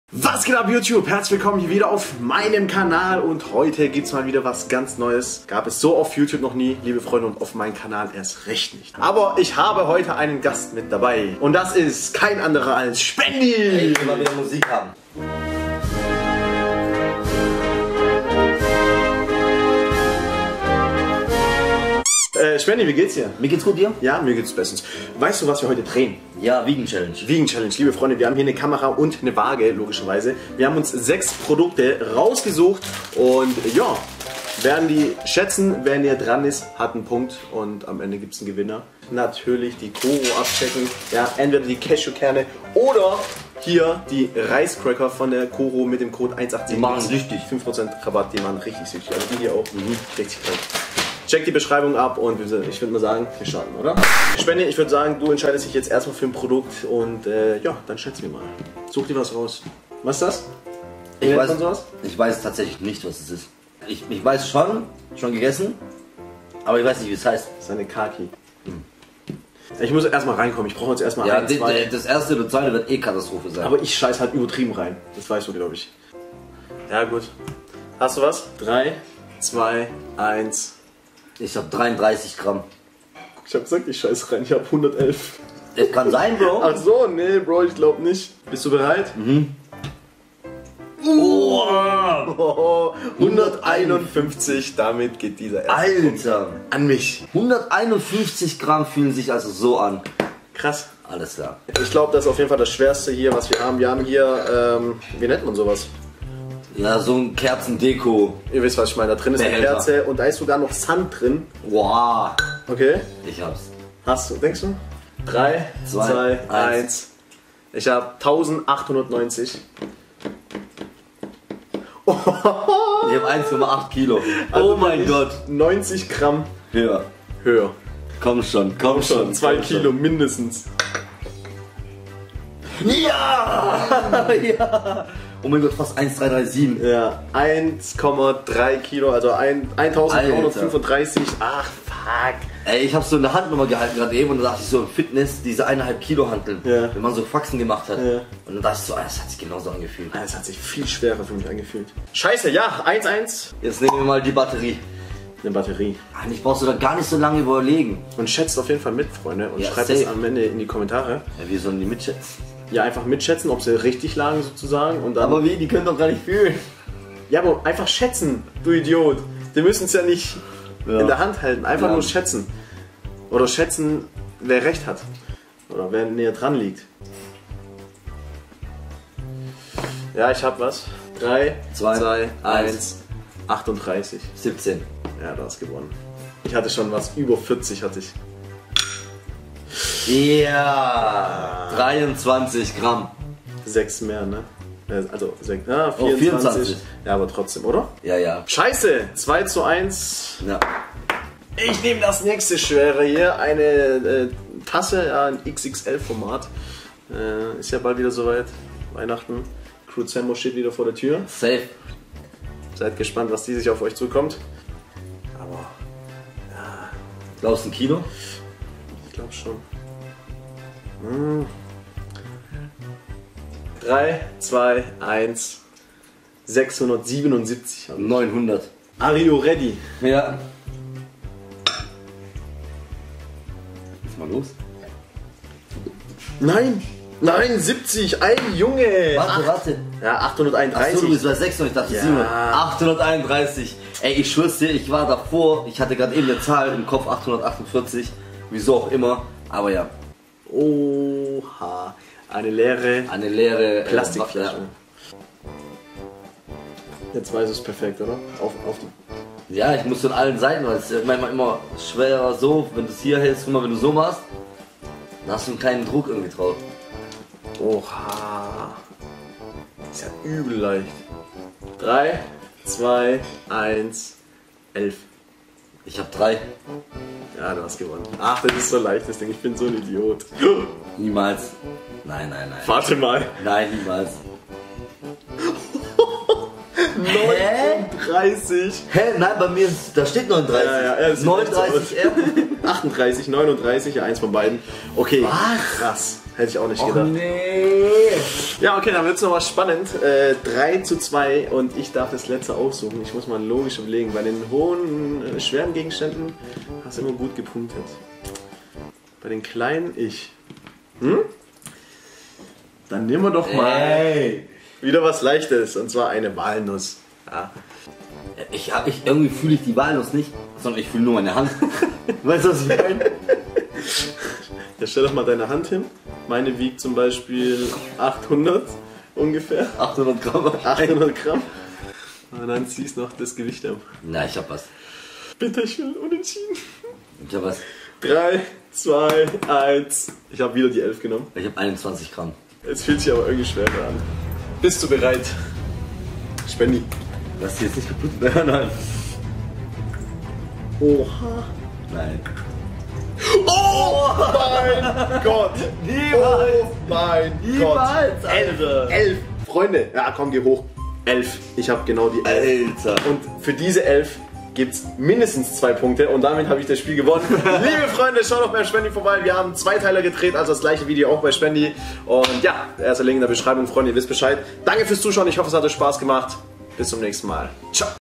Was wiegt Shpendis Ehre?
Genau das wollen alle wissen oder? Wie viel wiegt Shpendis Ehre? :D Das und noch vieles mehr wiegen wir heute im Video viel Spaß!